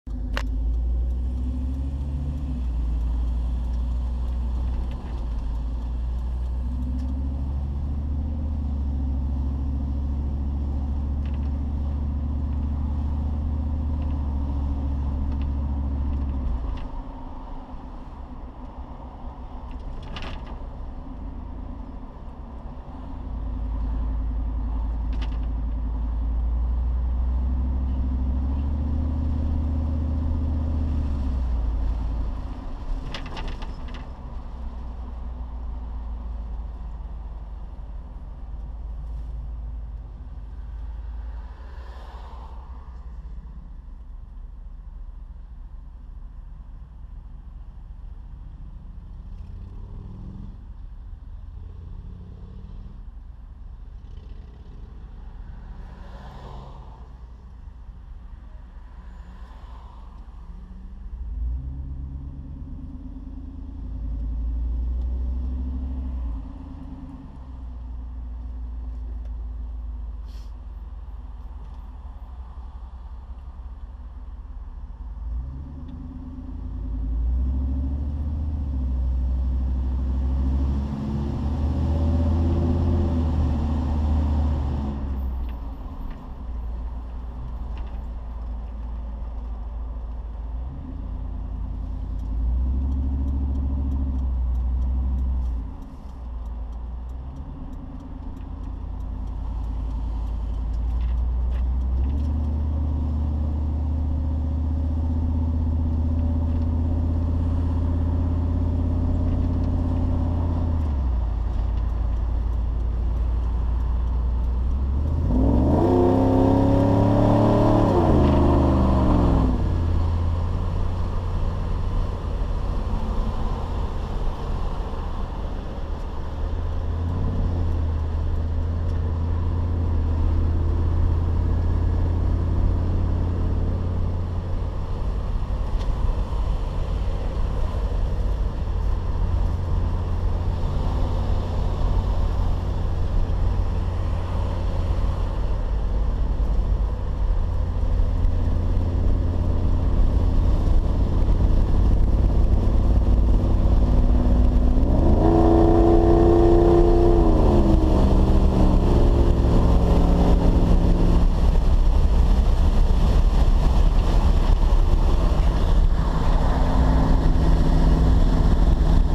S10 Crew Cab V8 350 Vortec Swapped Complete!
Here is a short driving video of my crew with the vortec 350 swap complete. The exhaust is installed. Driving with the windows down using my Go Pro camera.

Exhaust is Sanderson shorty headers to a Magnaflow High flow cat, 3 inch in and out to a magnaflow performance 3 inch single in out muffler. Custom 2.5- 3 inch Y pipe.